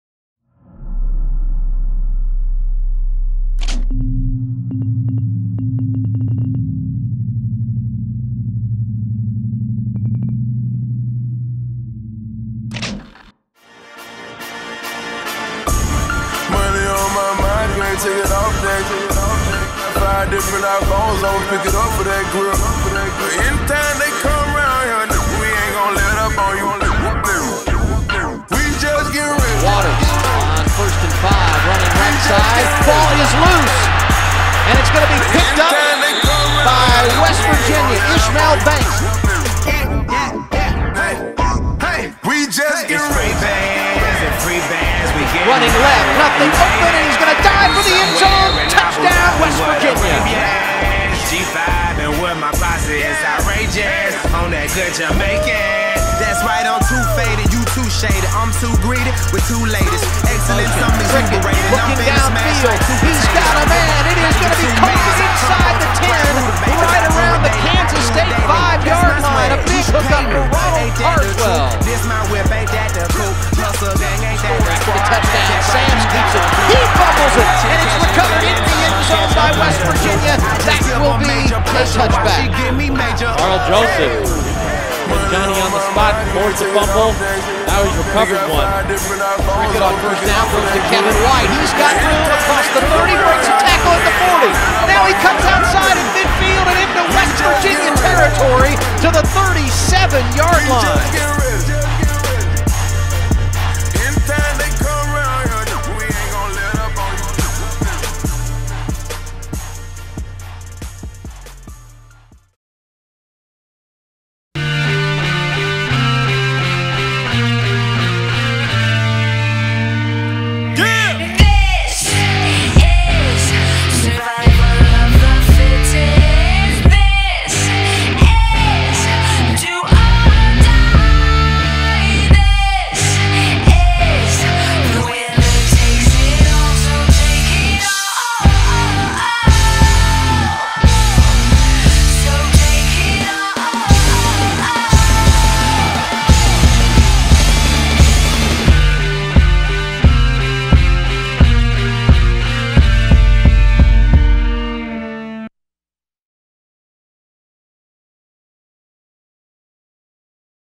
WVU Football 2013 Chapter 9: Them Or Us
Pre-game video for WVU matchup vs.TCU on 11/02/2013